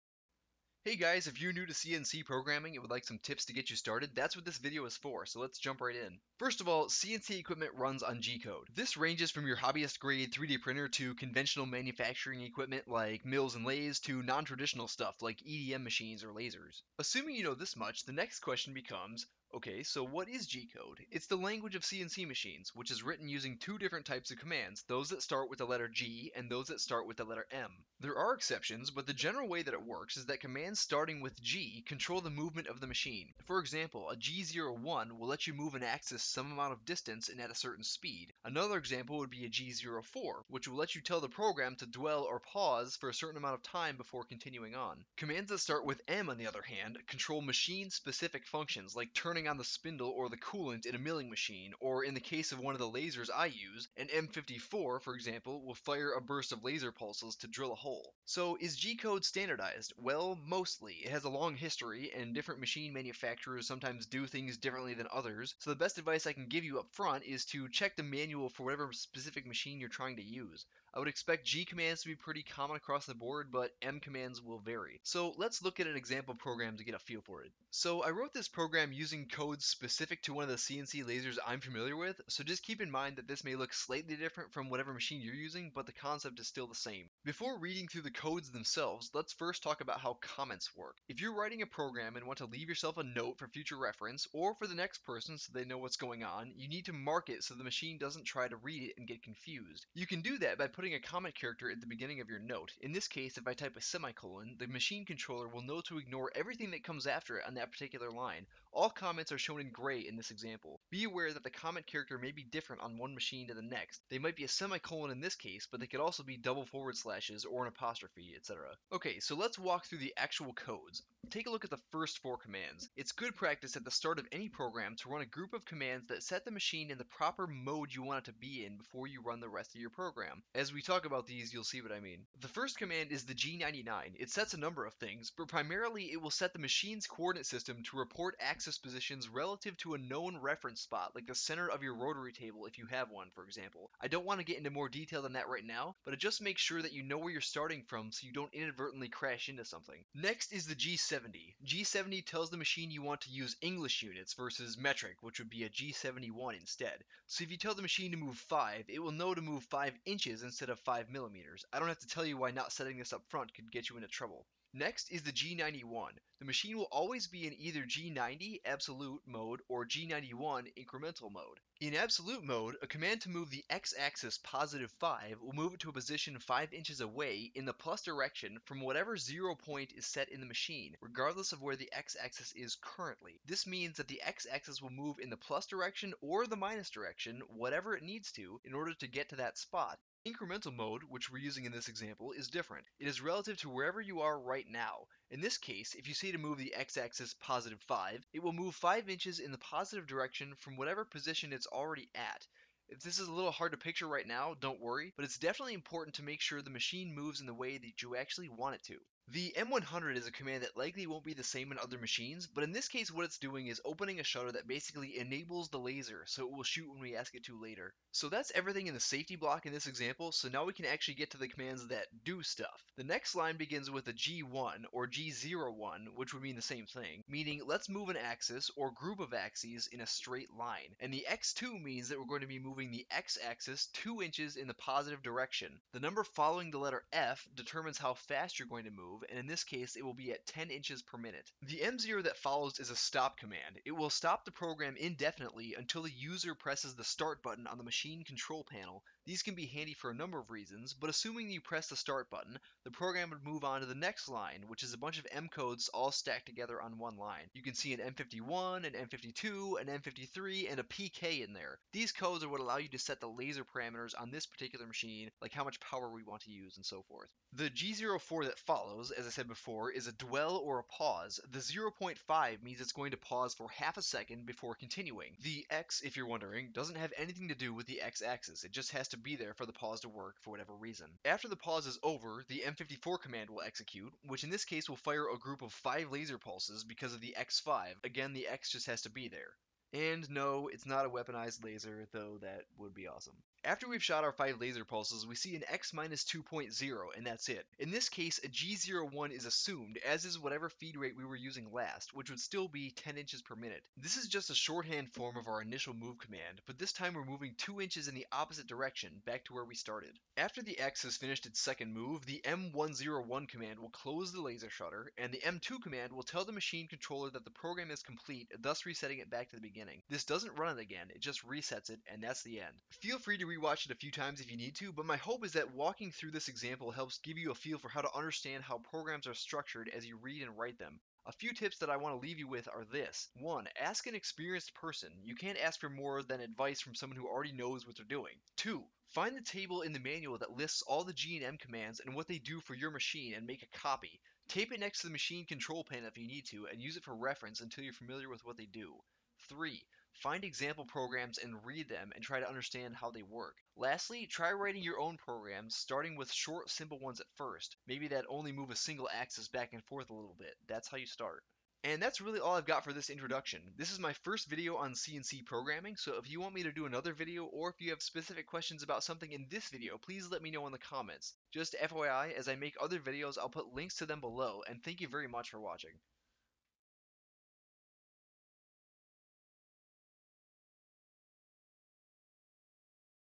⚙️⚡⚡ Basic Intro to CNC programming
This is a basic introduction to G-code programming for CNC machines.

This tutorial will give you the basics for learning to program all kinds of machines, from hobby-grade sheet cutters up to professional-grade mills and lathes.

Please ask questions in the comments!

⏰⏰⏰ Timestamps ⏰⏰⏰
0:00 - Intro
0:18 - What is G-code?
0:58 - Is G-code standardized?
1:15 - Example program
5:30 - Tips for learning G-code programming